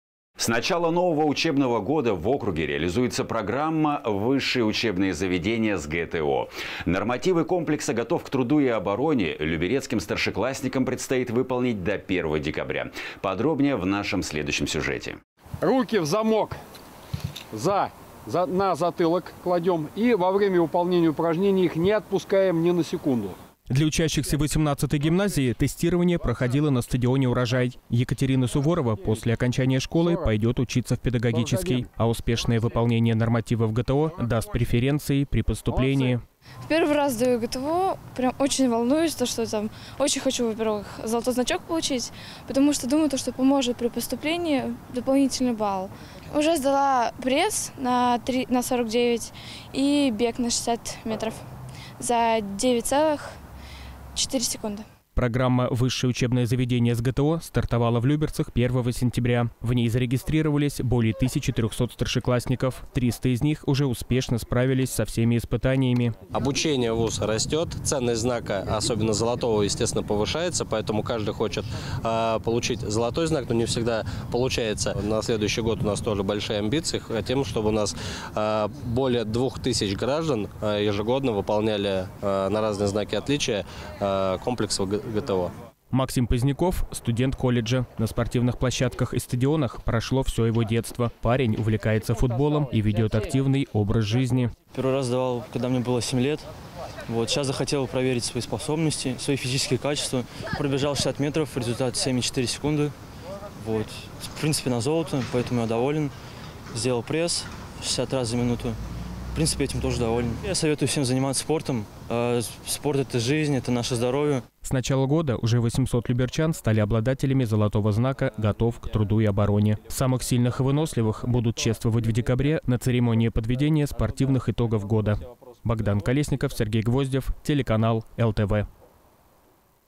Старшеклассники сдают нормативы ГТО
С начала нового учебного года в округе реализуется программа «В высшее учебное заведение с ГТО». Нормативы комплекса «Готов к труду и обороне» люберецким старшеклассникам предстоит выполнить до 1 декабря. Подробнее – в нашем следующем сюжете.

Для учащихся 18-й гимназии тестирование проходило на стадионе «Урожай». Екатерина Суворова после окончания школы пойдёт в педагогический. А успешное выполнение нормативов ГТО даст преференции при поступлении.

Екатерина Суворова, ученица гимназии № 18 г.о. Люберцы: «Первый раз сдаю ГТО. Очень волнуюсь. Очень хочу золотой значок получить, потому что поможет при поступлении, даст дополнительный балл. Уже сдала пресс на 49 и бег на 60 метров за 9,4 секунды».

Программа «В высшее учебное заведение с ГТО» стартовала в Люберцах 1 сентября. В ней зарегистрировались более 1300 старшеклассников. 300 из них уже успешно справились со всеми испытаниями.

Евгений Петров, руководитель Центра тестирования ГТО г.о. Люберцы: «Обучение в вуз растёт, ценность знака, особенного золотого, естественно, повышается. Поэтому каждый хочет получить золотой знак, но не всегда получается. На следующий год у нас тоже большие амбиции. Хотим, чтобы у нас более 2000 граждан ежегодно выполняли на разные знаки отличия комплекс ГТО».

Максим Поздняков – студент колледжа. На спортивных площадках и стадионах прошло всё его детство – парень увлекается футболом и ведёт активный образ жизни.

Максим Поздняков, житель г.о. Люберцы: «Первый раз сдавал, когда мне было 7 лет. Сейчас захотел проверить свои способности, свои физические качества. Пробежал 60 метров, результат 7,4 секунды. В принципе на «золото», поэтому я доволен. Сделал пресс 60 раз за минуту. В принципе этим тоже доволен. Я советую всем заниматься спортом. Спорт – это жизнь, это наше здоровье».

С начала года уже 800 люберчан стали обладателями золотого знака «Готов к труду и обороне». Самых сильных и выносливых будут чествовать в декабре на церемонии подведения спортивных итогов года.